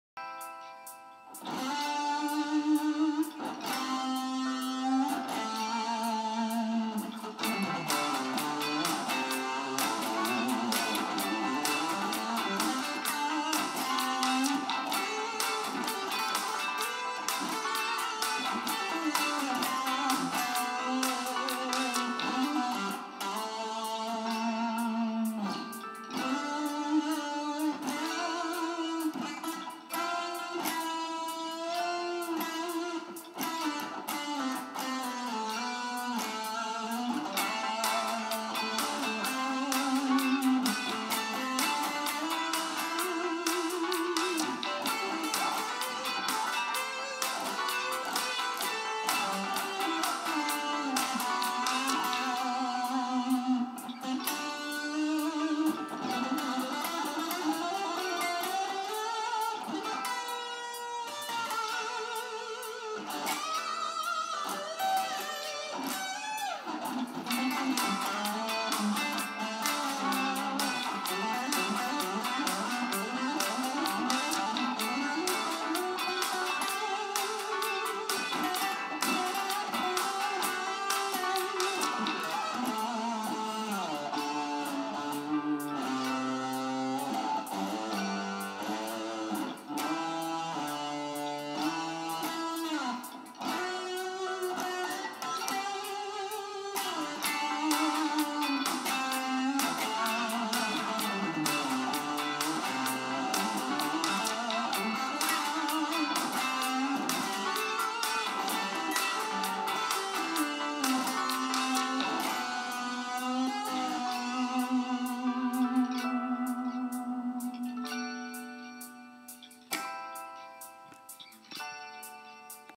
Guitar Improvisation 14 Years Old.
its in the key of A minor and i used a backing track from JamCenter.com